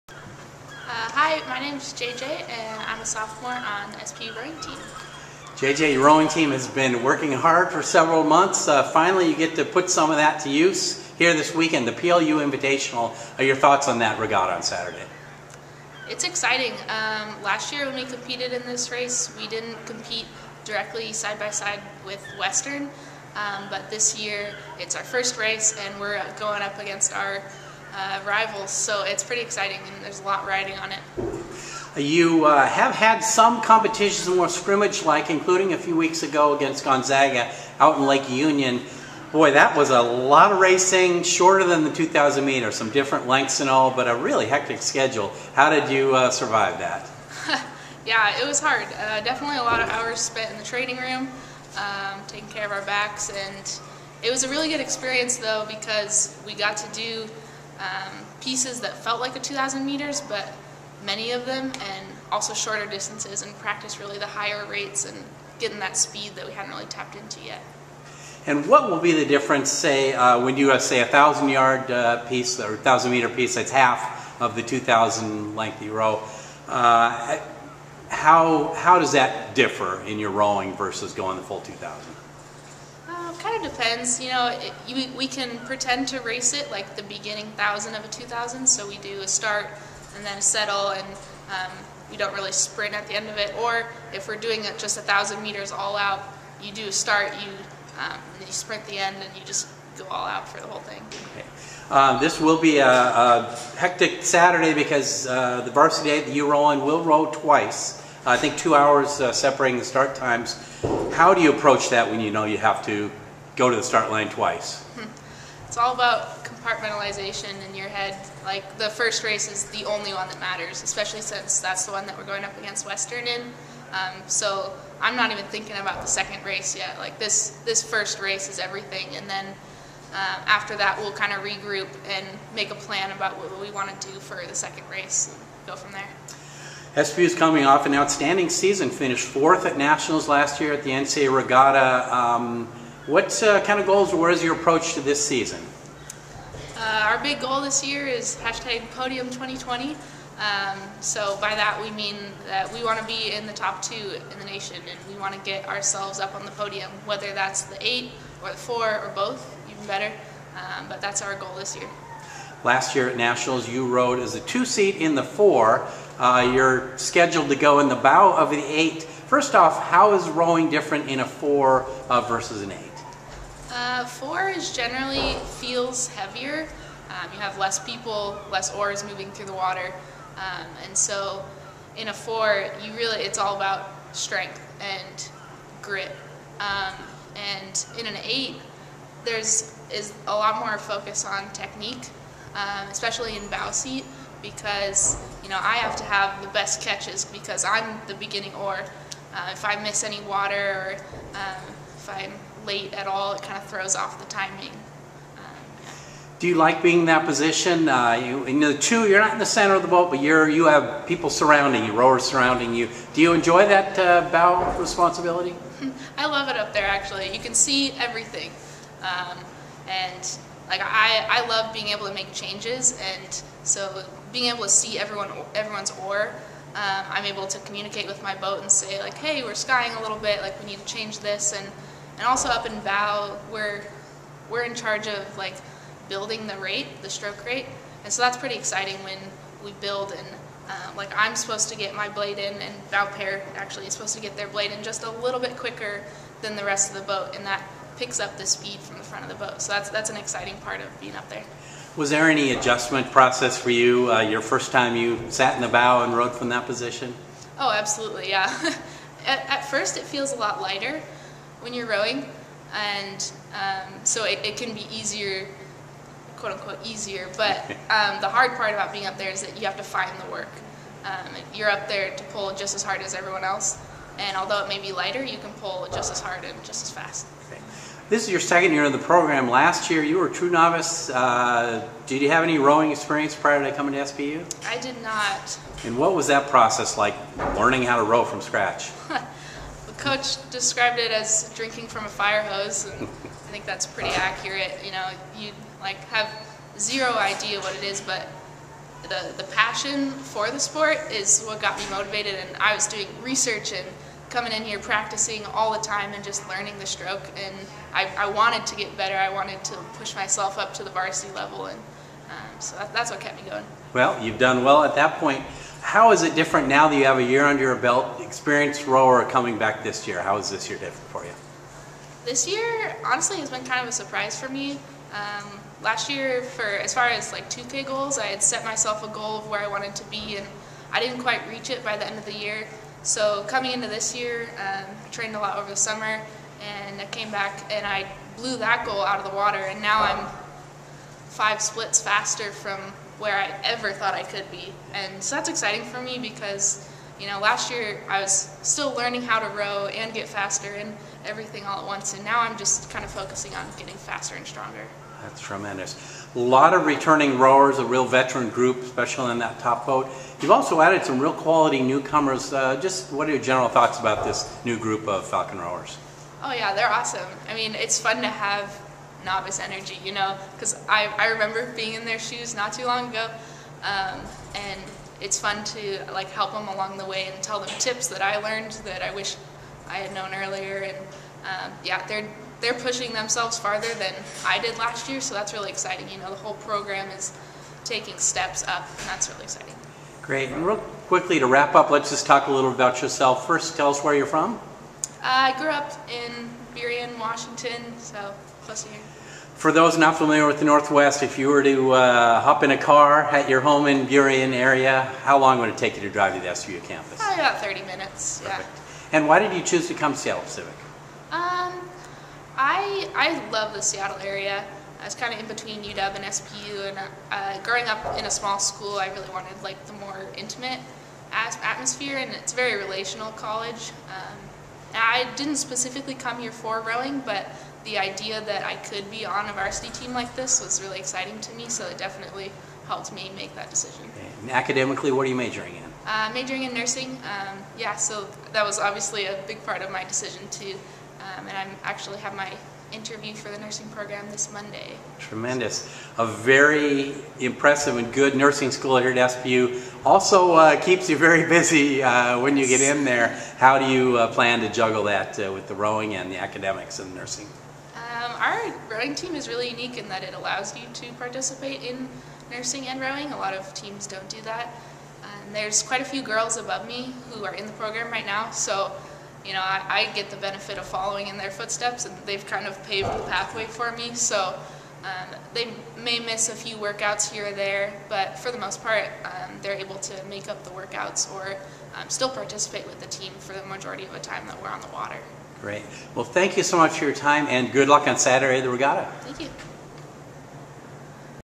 SPU ROWING: Jennifer Hoag (Mar. 5, 2020)
Seattle Pacific sophomore rower Jennifer Hoag talks about this Saturday's PLU Invitational that starts the 2020 spring racing season. She discusses why she chose to attend SPU, the process of learning to row and adjusting to her new position in the bow seat of the varsity eight crew.